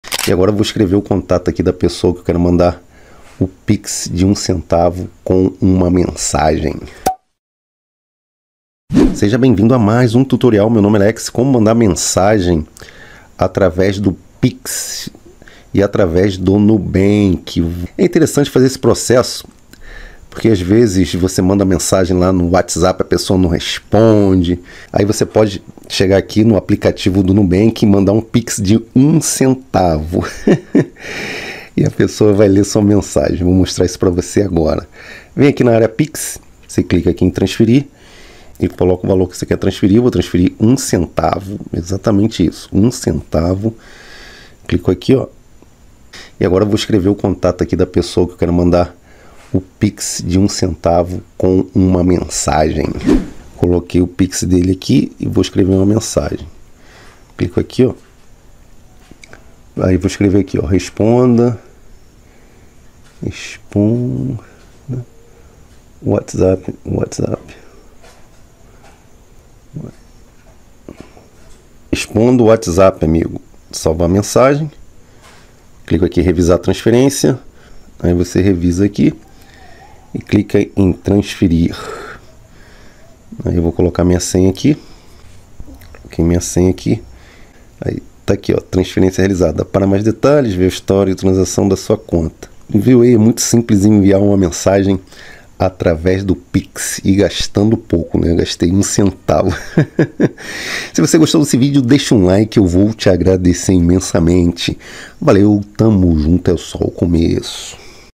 ⍇ Como Mandar Mensagem Atravez do Pix Nubank ➤ PIX Enviando Mensagens Personalizadas
Para enviar uma mensagem personalizada através do Pix no Nubank, siga estes passos simples:

Abra o aplicativo Nubank em seu dispositivo móvel.
Acesse a seção "Pix" no menu principal.
Selecione a opção "Enviar Pix".
Escolha o destinatário inserindo o CPF/CNPJ, e-mail, número de telefone ou chave Pix do destinatário.
Após inserir o valor a ser enviado, você verá um campo para "Mensagem" ou "Descrição". Digite sua mensagem personalizada aqui.
Verifique todas as informações, incluindo o valor e a mensagem, e confirme o envio.PixNubank,
PagamentosInstantâneos,
TransferênciasBancárias,

Pronto! Sua mensagem personalizada será enviada juntamente com a transação Pix. Certifique-se de que o destinatário esteja ciente de que você incluiu uma mensagem para evitar confusões.

▬▬▬▬▬▬▬▬▬▬▬ஜ۩۞۩ஜ▬▬▬▬▬▬▬▬▬▬▬▬▬
▓▓▓▒▒▒░░░ Abaixo Link relacionado ao vídeo  ░░░▒▒▒▓▓▓
▬▬▬▬▬▬▬▬▬▬▬ஜ۩۞۩ஜ▬▬▬▬▬▬▬▬▬▬▬▬▬

Nosso Canal de Receitas Vejetarianas e Veganas

▬▬▬▬▬▬▬▬▬▬▬ஜ۩۞۩ஜ▬▬▬▬▬▬▬▬▬▬▬▬▬

Fundo musical retirado deste site